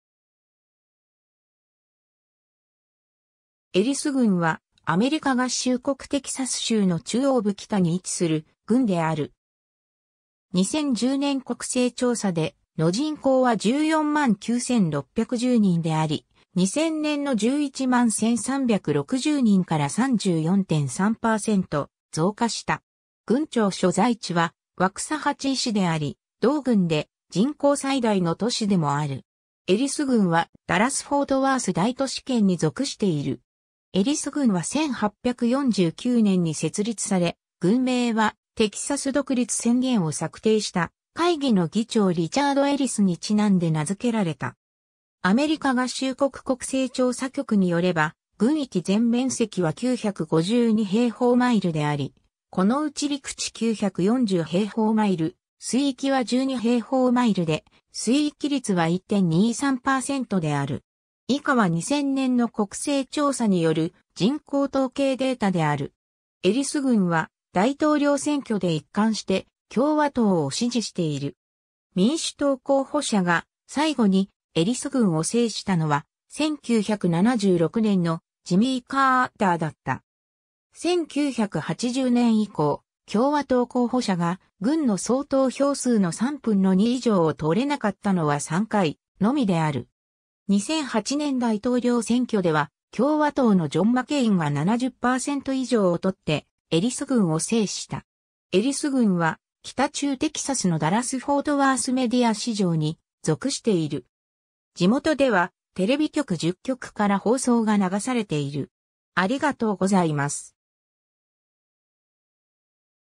エリス郡 (テキサス州)
エリス郡（英: Ellis County）は、アメリカ合衆国テキサス州の中央部北に位置する郡である。
2010年国勢調査での人口は149,610人であり、2000年の111,360人から34.3%増加した。
郡庁所在地はワクサハチー市（人口29,621人）であり、同郡で人口最大の都市でもある。
エリス郡はダラス・フォートワース大都市圏（メトロプレックスと呼ばれている）に属している。
エリス郡は1849年に設立され、郡名はテキサス独立宣言を策定した会議の議長リチャード・エリスに因んで名付けられた。
 エリス郡は大統領選挙で一貫して共和党を支持している。
民主党候補者が最後にエリス郡を制したのは、1976年のジミー・カーターだった。
1980年以降、共和党候補者が郡の総投票数の3分の2以上を取れなかったのは3回のみである。
2008年大統領選挙では、共和党のジョン・マケインが70%以上を取ってエリス郡を制した。
 エリス郡は北中テキサスのダラス・フォートワース・メディア市場に属している。
地元ではテレビ局10局から放送が流されている。